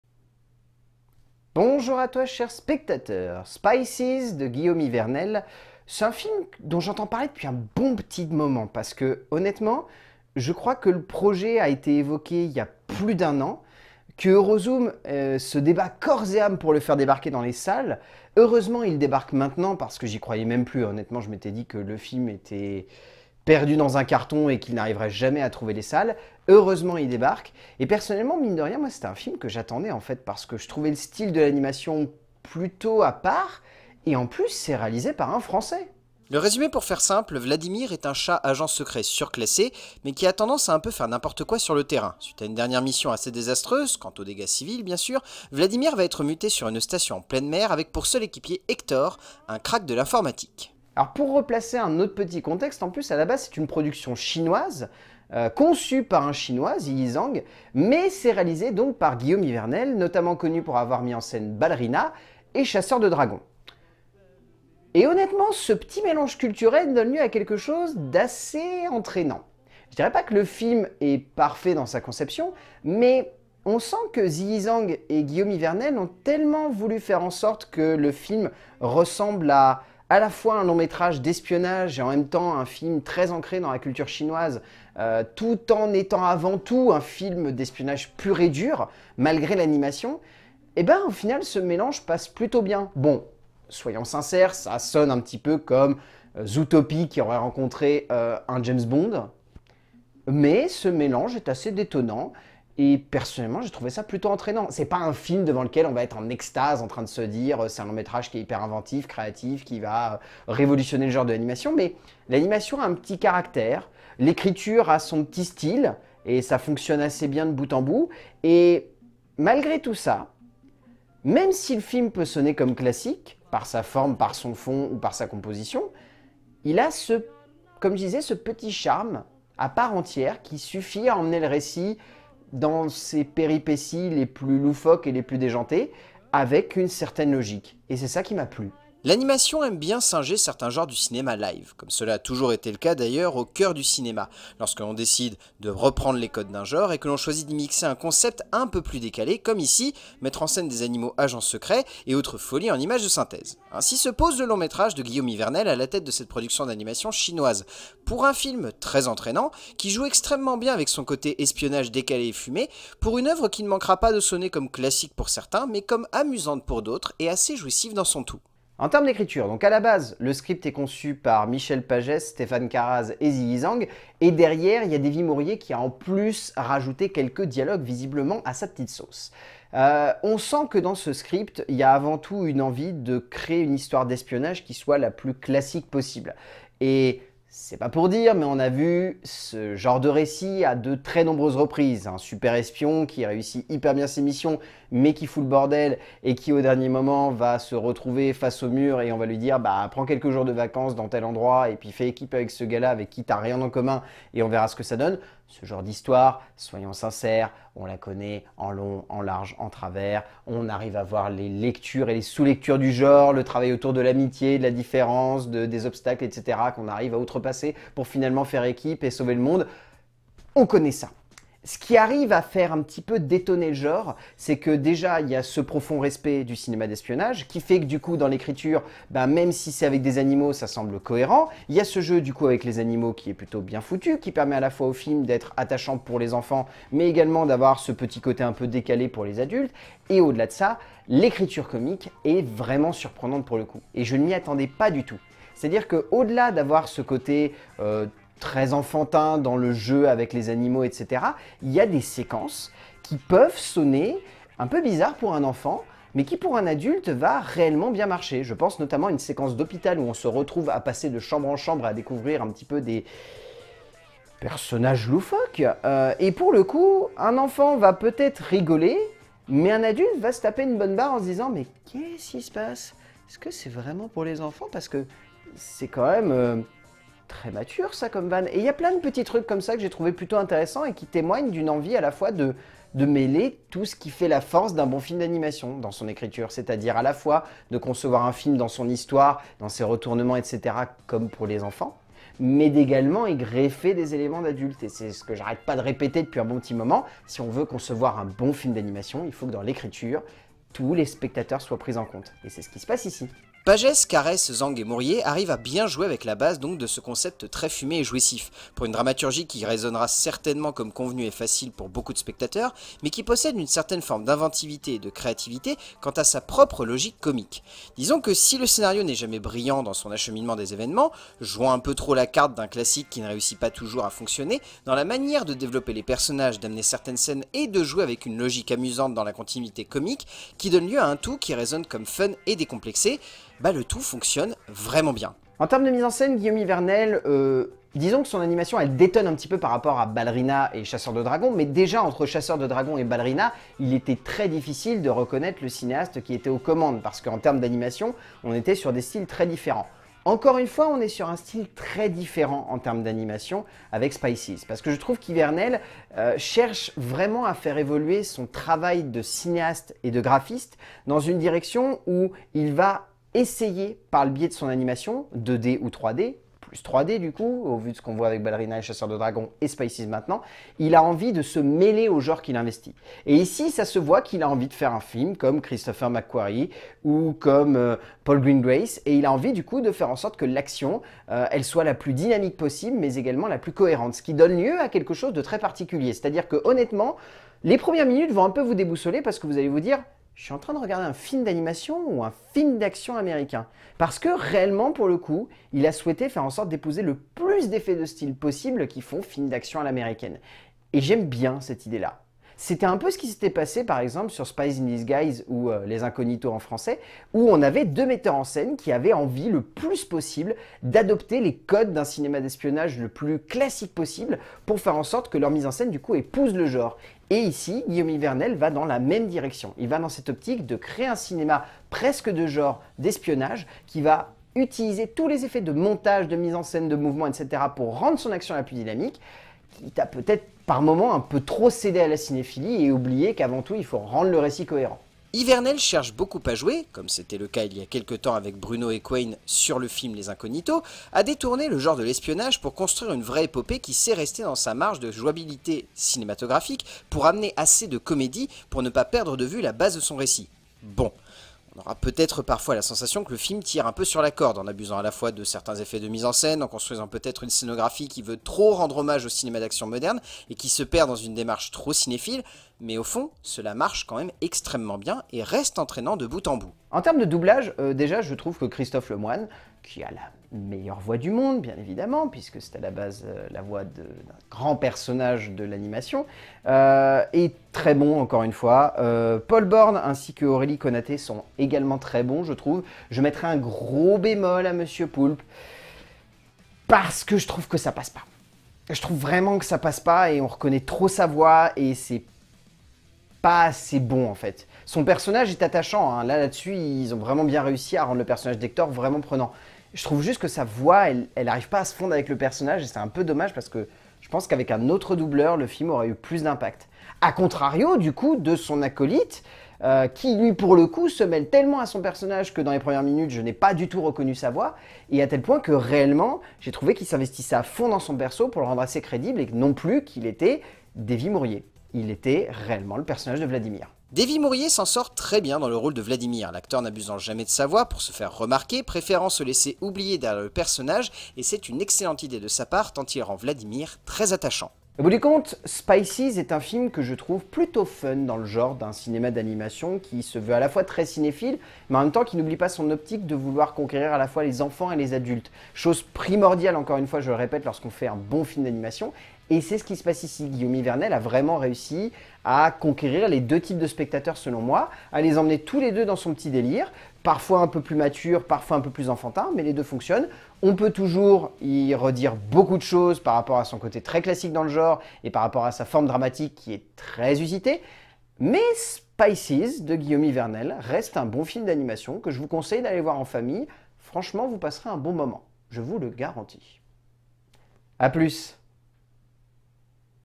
LCDC - Spycies
Chronique critique sur la sortie d'un film d'animation pas forcément extraordinaire dans sa construction, mais très inventif dans son animation, et son mélange des genres, "Spycies"